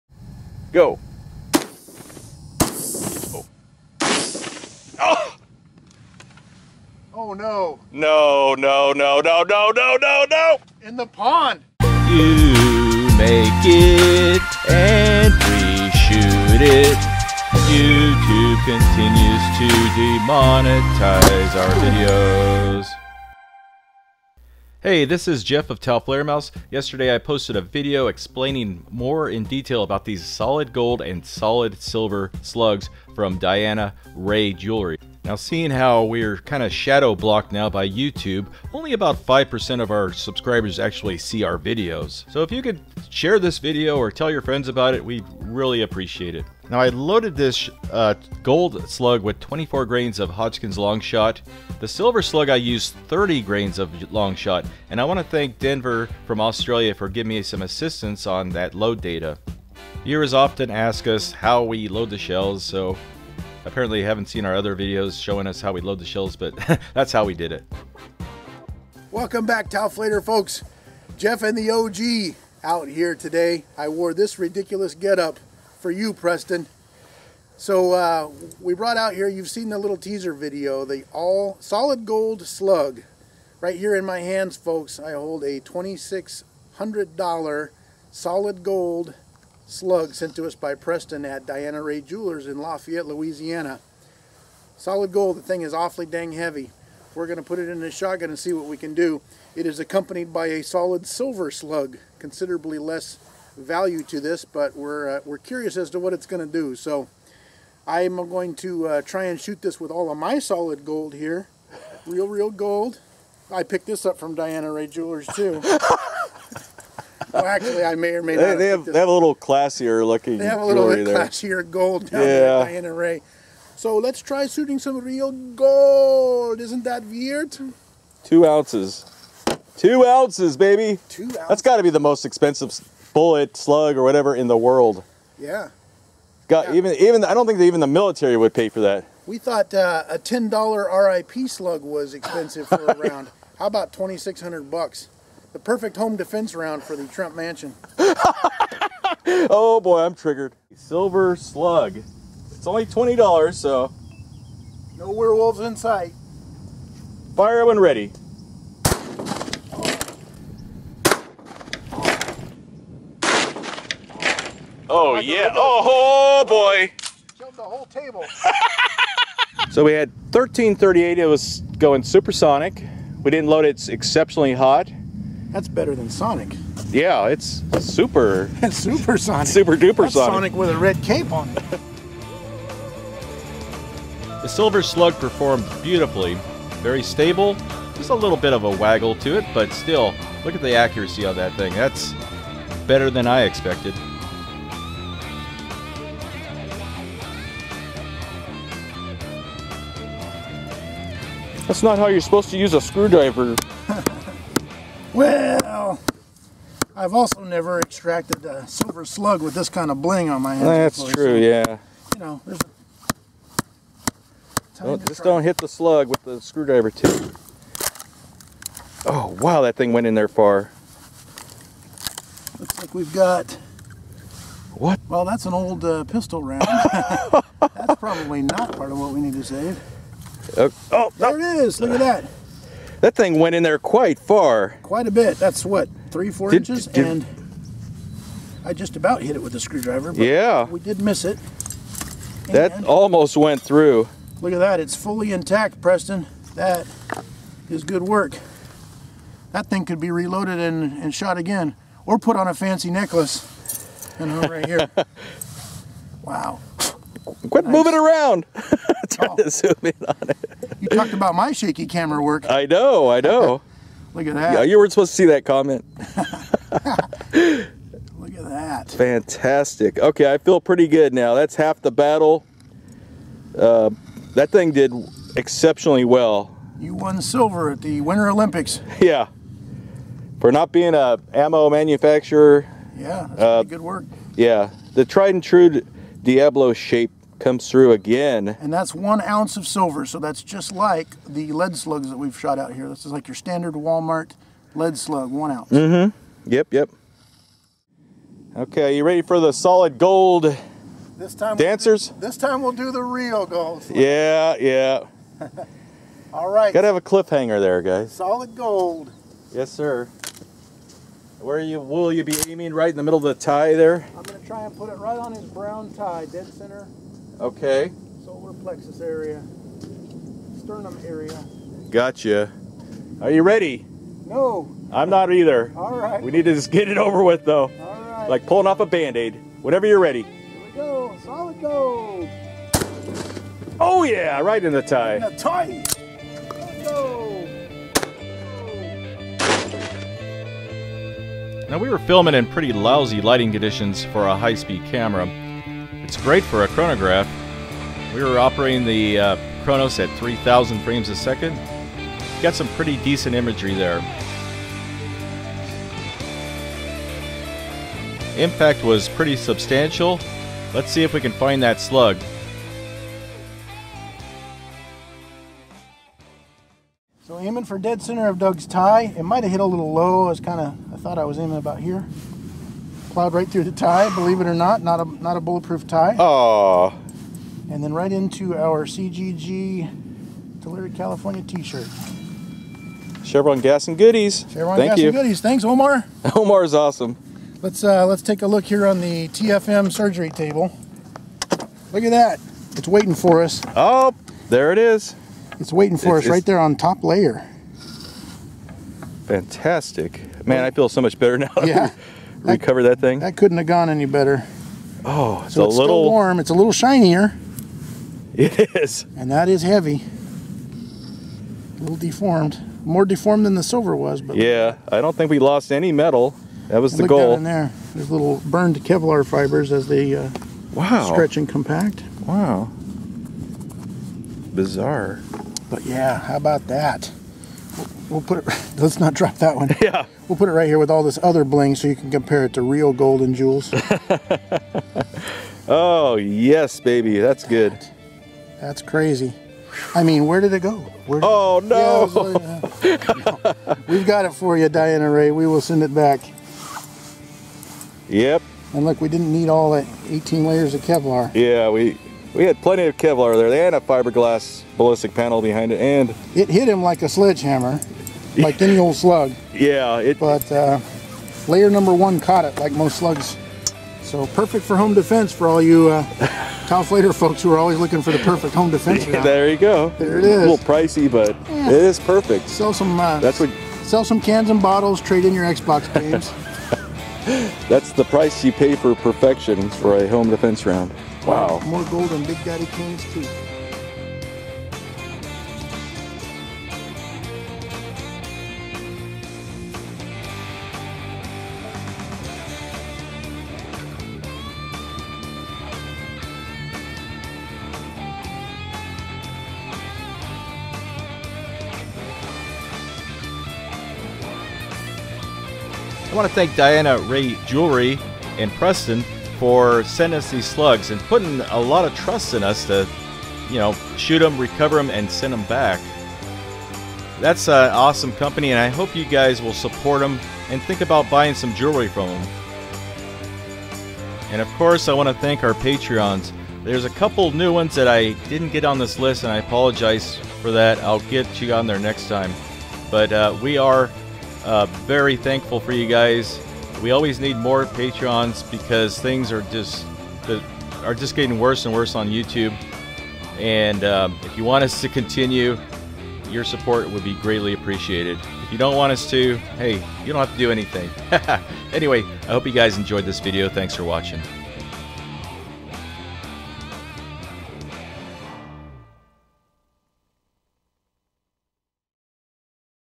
World's MOST EXPENSIVE Shotgun Slug - 2 oz. SOLID GOLD
In this video we shoot the most expensive bullet in the world,  a 2 oz. solid gold slug.   As a bonus,  we also shoot a solid silver slug too.   This was a once in a lifetime experience and I hope you enjoy the video as much as we enjoyed making it.   We recovered the gold and silver slugs  and returned them to the jewelers who trusted us with them.

 Our store location is:
500 Settlers Trace Boulevard
Suite 1